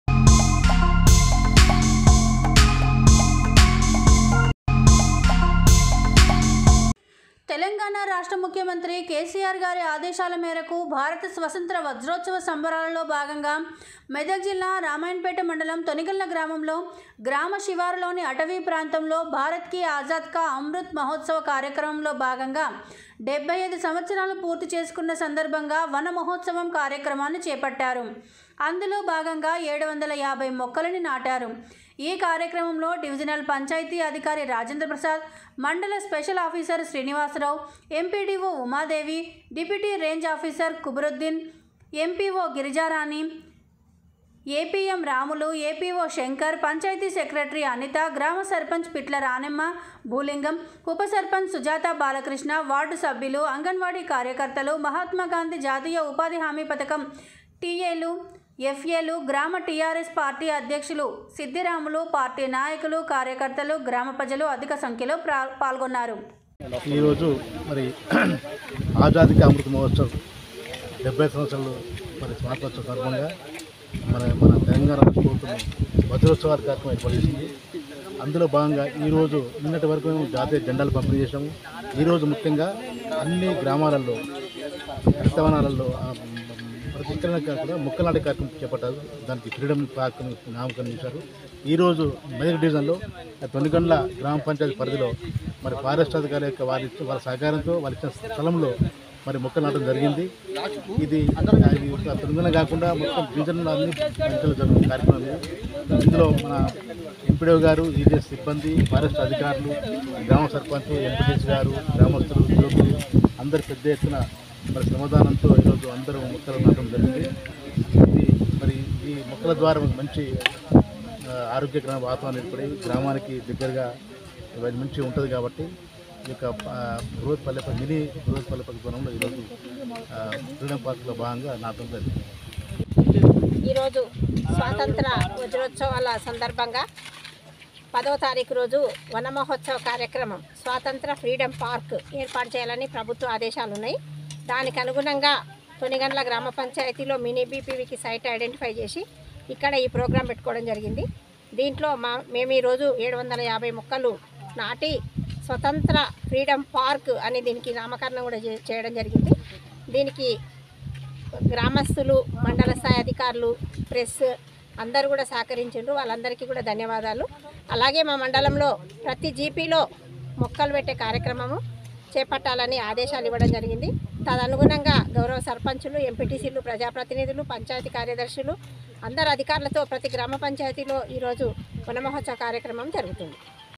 తొనిగండ్ల గ్రామంలో గ్రామ శివారులోని అటవీ ప్రాంతంలో భారత్ కి ఆజాద్ క అమృత్ మహోత్సవ్ కార్యక్రమం
మెదక్ జిల్లా రామాయంపేట మండలం తొనిగండ్ల గ్రామంలో గ్రామ శివారులోని అటవీ ప్రాంతంలో భారత్ కి ఆజాద్ క అమృత్ మహోత్సవ్ కార్యక్రమంలో భాగంగా 75 సంవత్సరాలు పూర్తి చేసుకున్న సందర్భంగా వన మహోత్సవం కార్యక్రమాన్ని చేపట్టారు